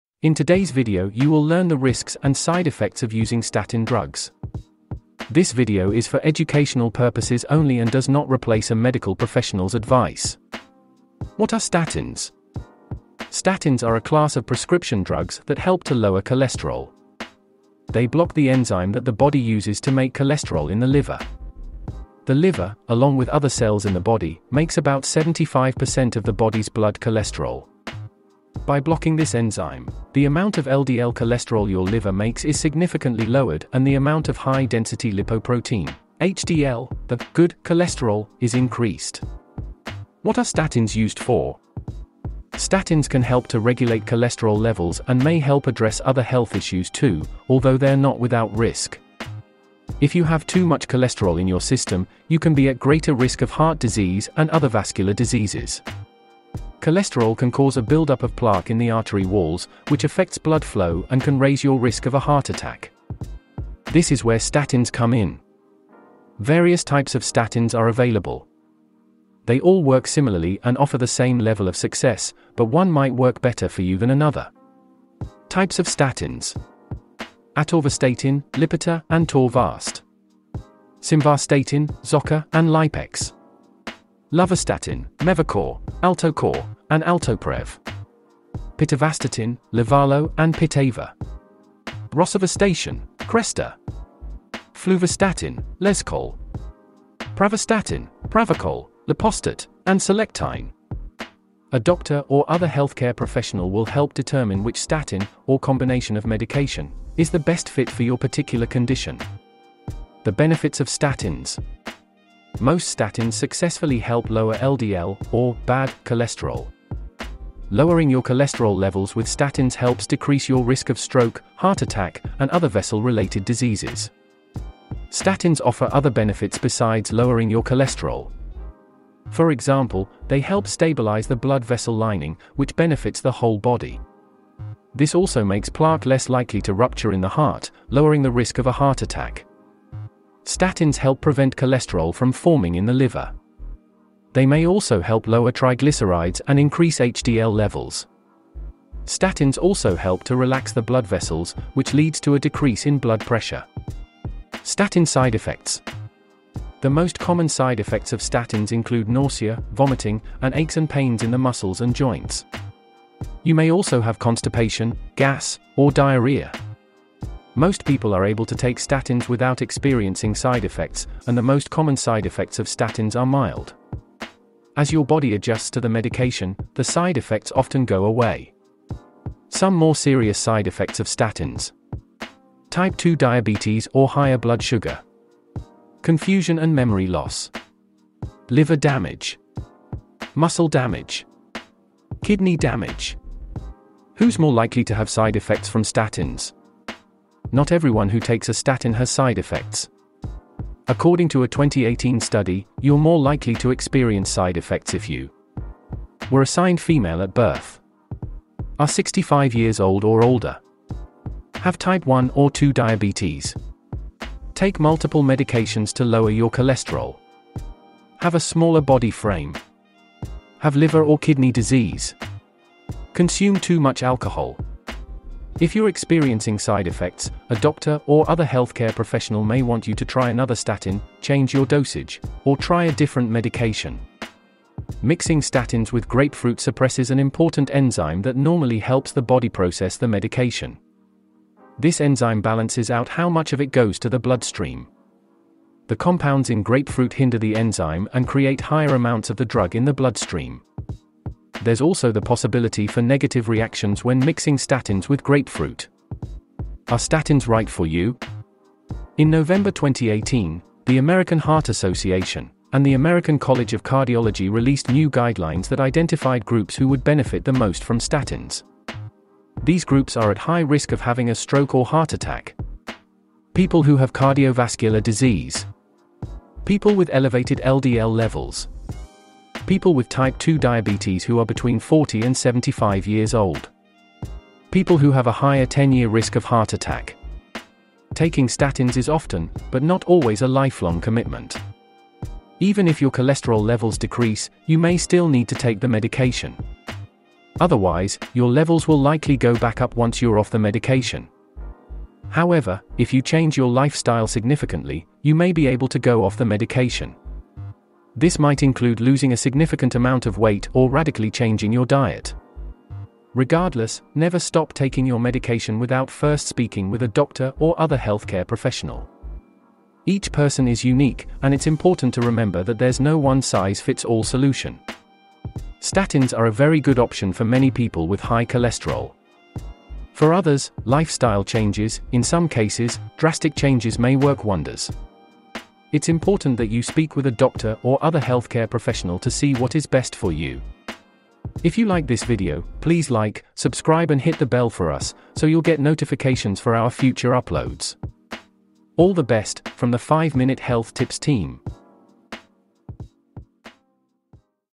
Dangers and Benefits of Cholesterol Drugs for Seniors (VERIFIED)
In this video, you will learn the Dangers and Benefits of Cholesterol Drugs for Seniors (Statin Drugs).
Statins are a class of prescription drugs that help to lower cholesterol. They block the enzyme that the body uses to make cholesterol in the liver. The liver, along with other cells in the body, makes about 75 percent of the body’s blood cholesterol. By blocking this enzyme, the amount of LDL cholesterol your liver makes is significantly lowered and the amount of high-density lipoprotein (HDL), the “good” cholesterol, is increased..

Each person is unique, and it’s important to remember that there’s no one-size-fits-all solution. Statins are a very good option for many people with high cholesterol. For others, lifestyle changes (in some cases, drastic changes) may work wonders. It’s important that you speak with a doctor or other healthcare professional to see what is best for you.

if you like this video, please like, subscribe and hit the bell for us, so you’ll get notifications for our future uploads. all the best, From the 5 minute health tips team.